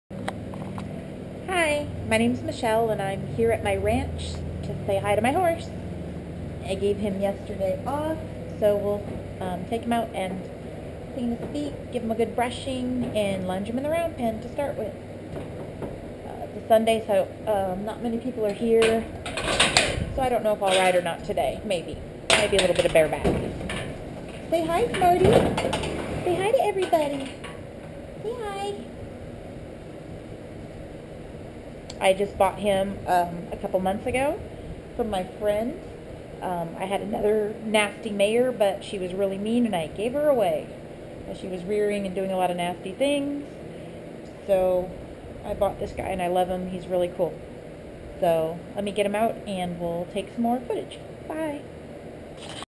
OK so I tried to talk into the camera at I guess too high of a quality, which caused the sound and video to beocme off sync.. oh well I will try again today with a lower setting .. I am such a gopro newb! :)